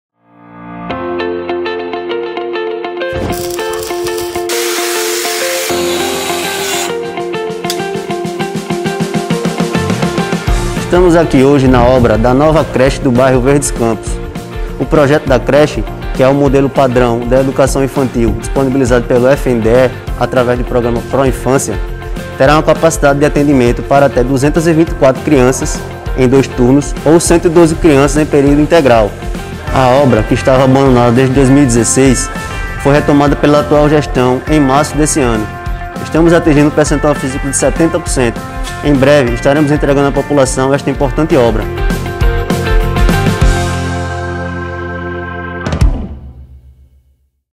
CRECHE DO BAIRRO VERDES CAMPOS
A espera acabou! 🙌🏻
Além de realizar o maior programa de pavimentação em Arapiraca, estamos investindo também na educação com reformas e construção de creches e escolas nas zonas rural e urbana. 🏫
A creche no bairro Verdes Campos é um exemplo. Abandonada na gestão passada, a creche do Verdes Campos está 70% concluída e, em breve, mais de 200 crianças e pais terão um espaço novo para educação e lazer. 🧍🏿‍♀️🧍🏽
É a Prefeitura fazendo Mais, Melhor e Do jeito certo! ✅